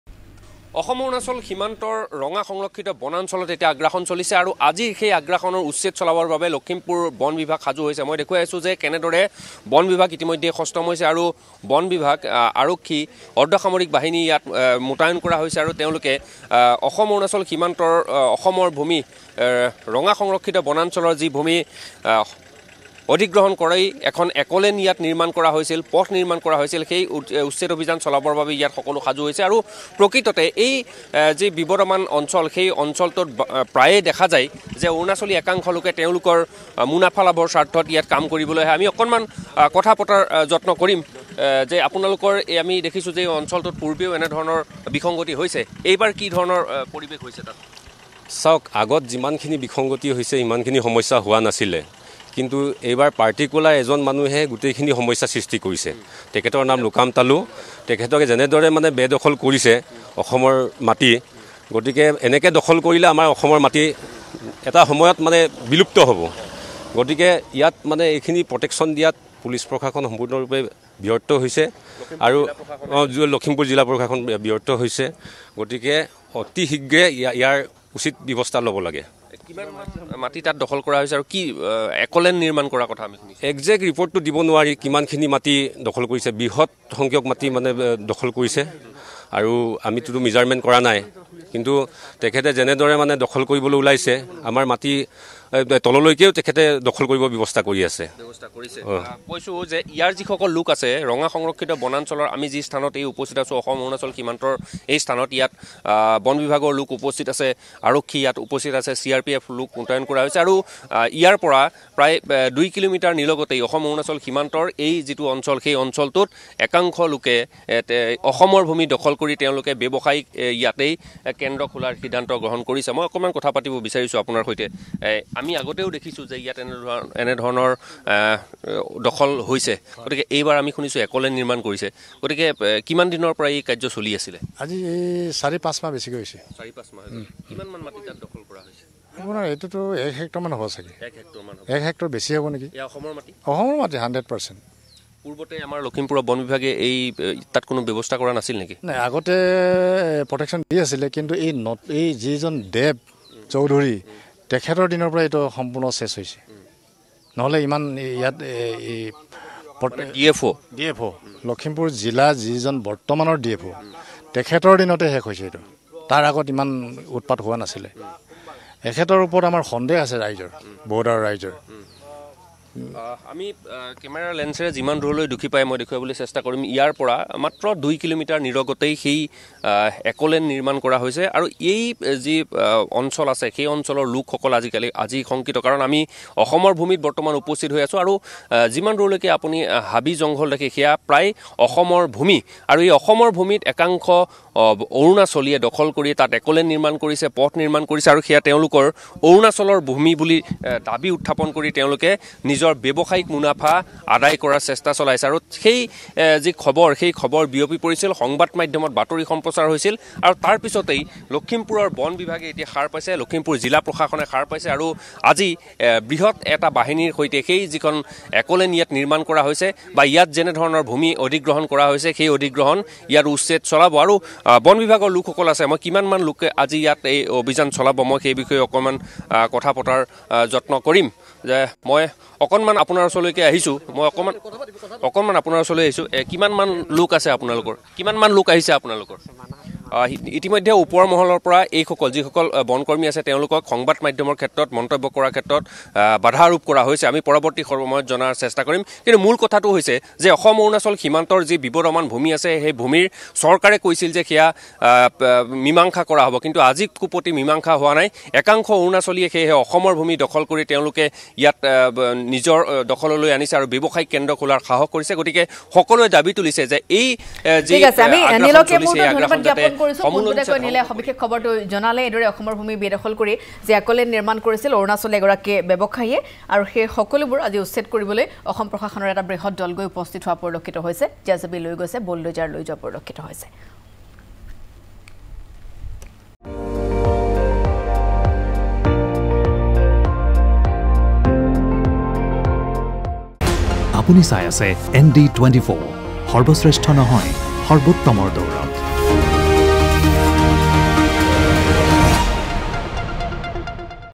Assam News: লখিমপুৰৰ ৰঙা সংৰক্ষিত বনাঞ্চলত চলিব উচ্ছেদ
লখিমপুৰৰ ৰঙা সংৰক্ষিত বনাঞ্চলত চলিব উচ্ছেদ। অৰুণাচলীয়ে দখল কৰি নিৰ্মাণ কৰা ঘৰ ভাঙিব জিলা প্ৰশাসনে। অবৈধভাৱে নিৰ্মাণ কৰা এক'লেণ্ড ভাঙি পেলোৱাৰ লগতে পথটো বন্ধ কৰি দিব লখিমপুৰ প্ৰশাসনে।

▶️ ৰাজপাট কাৰ?